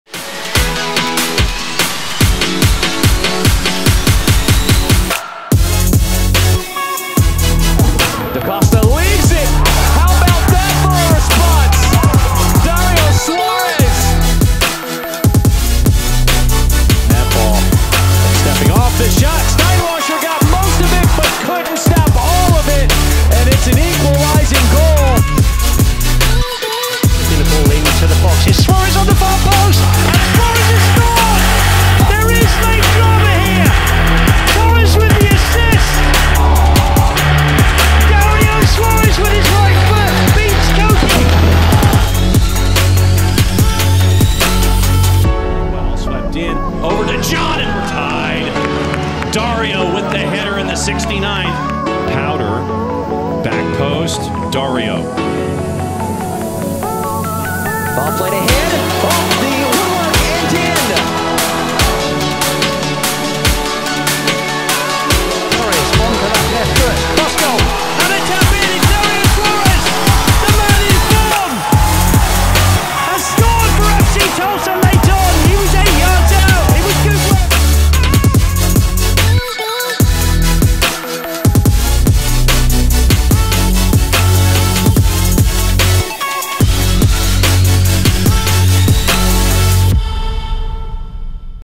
Darío Suárez is Back in Tulsa for the 2023 Season
Join Club 918 by purchasing your 2023 season tickets today!

View FC Tulsa's Entire 2023 Roster here!